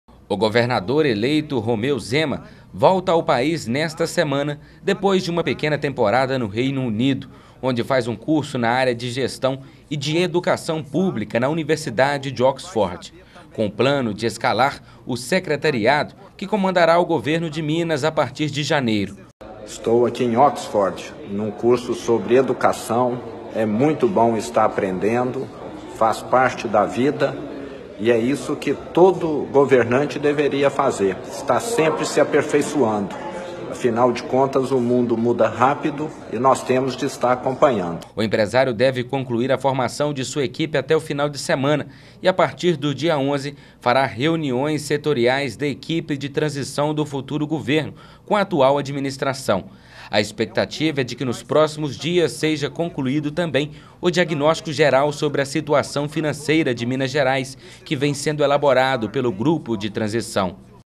Governador eleito Romeu Zema (Novo) volta ao país nesta semana
O governador eleito Romeu Zema (Novo) volta ao país nesta semana – depois de uma pequena temporada no Reino Unido, onde fez curso na área de gestão e de educação pública na Universidade de Oxford – com o plano de escalar o secretariado que comandará o governo de Minas a partir de janeiro. O empresário deve concluir a formação de sua equipe até o final da semana e, a partir do dia 11, fará reuniões setoriais da equipe de transição do futuro governo com a atual administração. A expectativa é de que nos próximos dias seja concluído também o diagnóstico geral sobre a situação financeira de Minas Gerais que vem sendo elaborado pelo grupo de transição.

Até agora, o governador eleito anunciou dois secretários de seu governo. Os primeiros nomes divulgados por Zema foram o do contador Gustavo Barbosa, que assumirá a Secretaria de Estado da Fazenda, e o do engenheiro Otto Alexandre Levy Reis, que ficará à frente da de Planejamento e Gestão. As duas pastas são consideradas estratégicas para a área econômica, que terá como desafio tirar o estado da grave crise fiscal.

Durante a campanha eleitoral, o empresário afirmou que sua gestão terá como marca a redução drástica da máquina pública. Zema prometeu reduzir o número de secretarias das atuais 21 pastas para apenas nove. No entanto, o número final pode chegar a 10 ou 11 secretarias, o que ainda está sendo discutido pelo futuro governador.